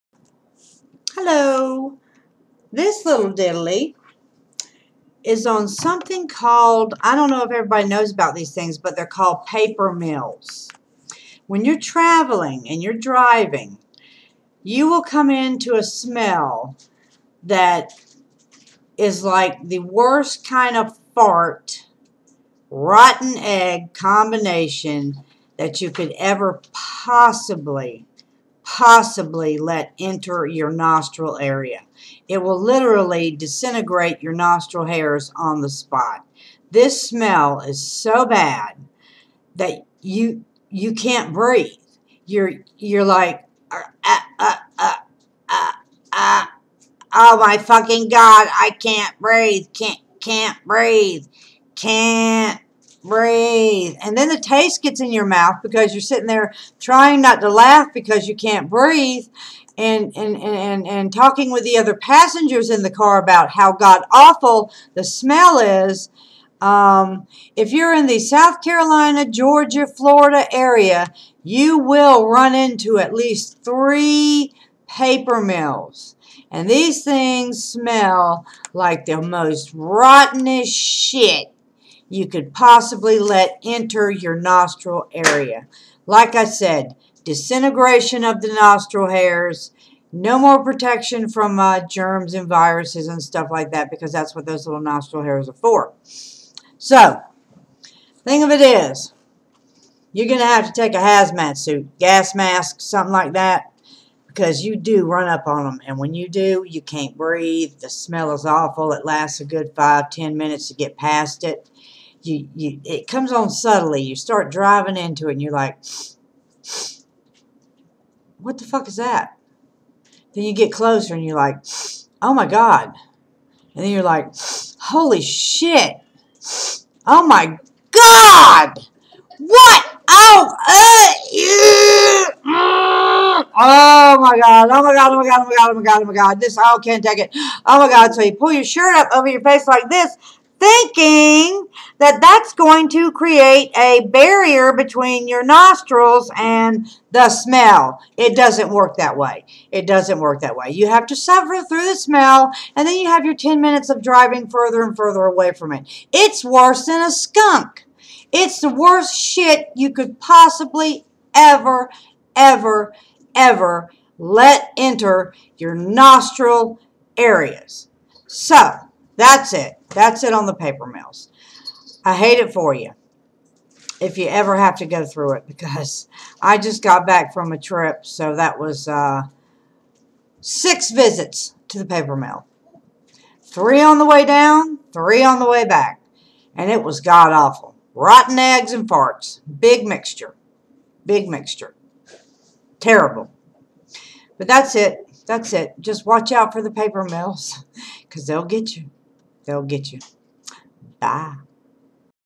paper mills and the smell...get a hazmat suit!!
the sick smell of paper mills is like farts and rotten eggs combined!!!